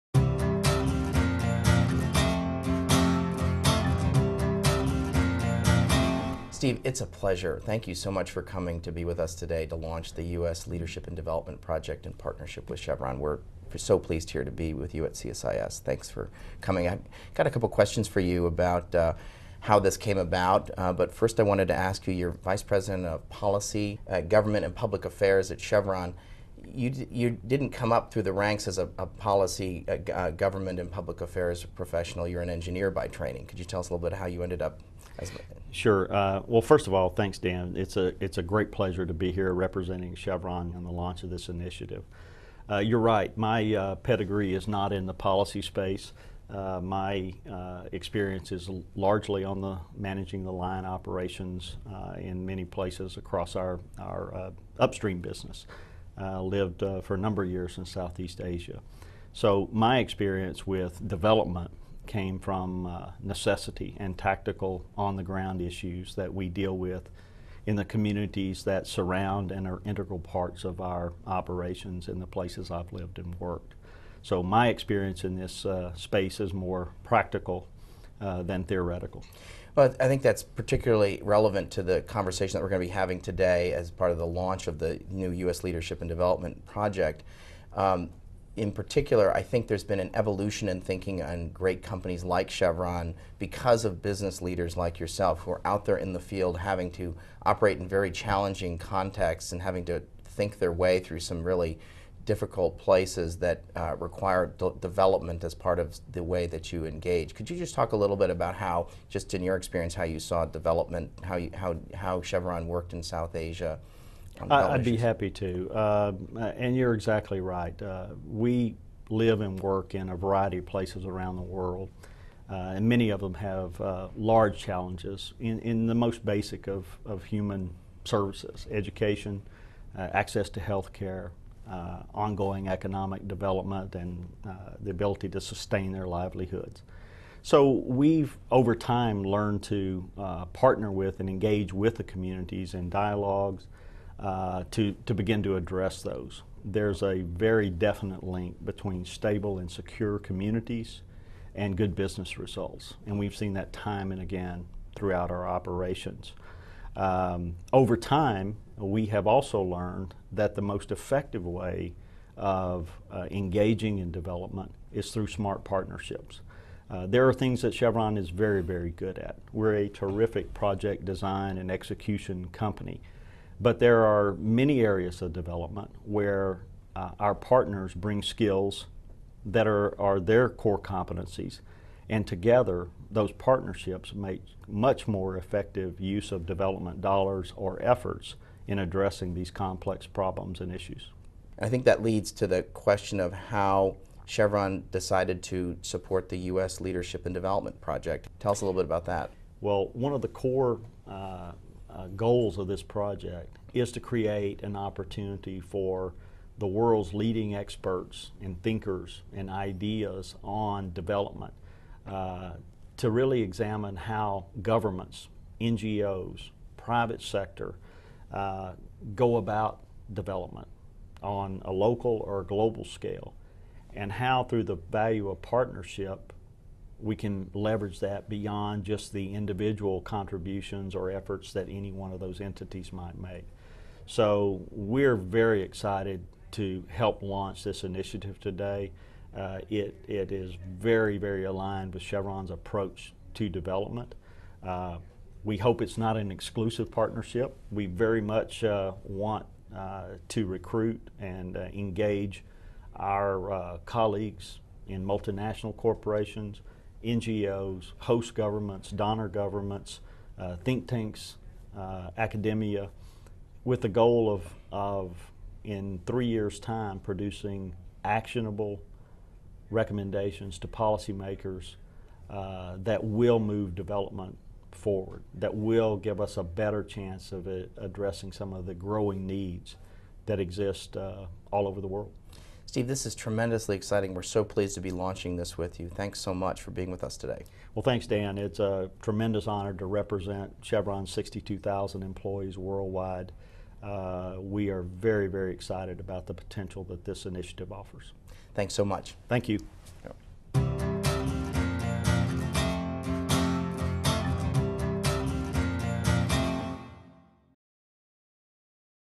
The Launch of the CSIS Chevron Project on U S Leadership in Development
Daniel Runde speaks with Stephen Green, Vice President of Chevron about the launch of the CSIS-Chevron Project on U.S. Leadership in Development.